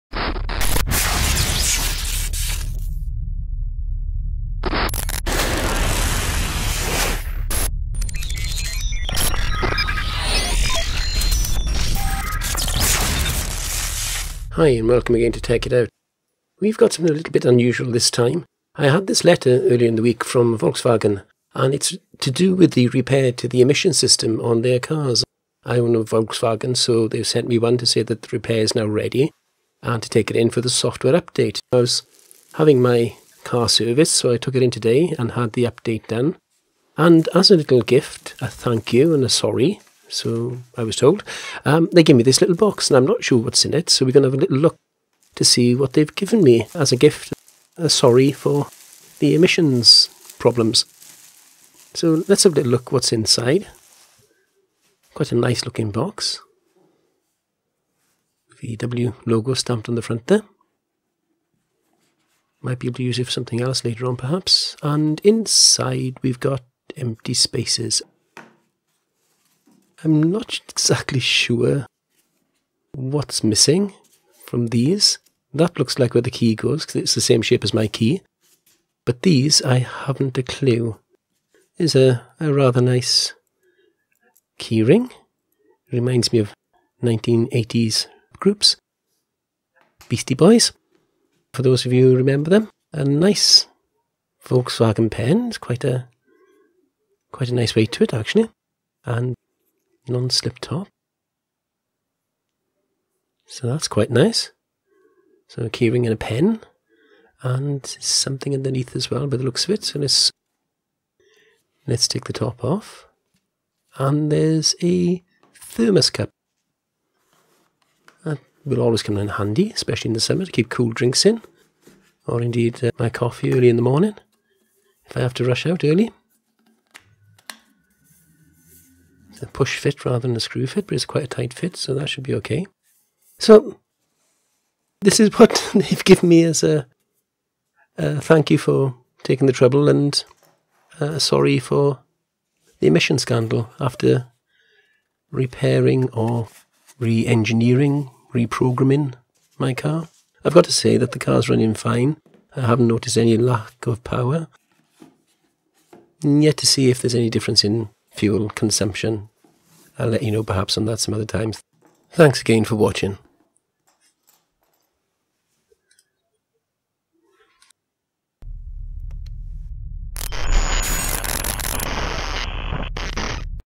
VW emissions scandal official fix. Gift unboxing.
Unboxing the gift I received today as a sorry for my cay being part of the scandal after having the software fix installed by VW.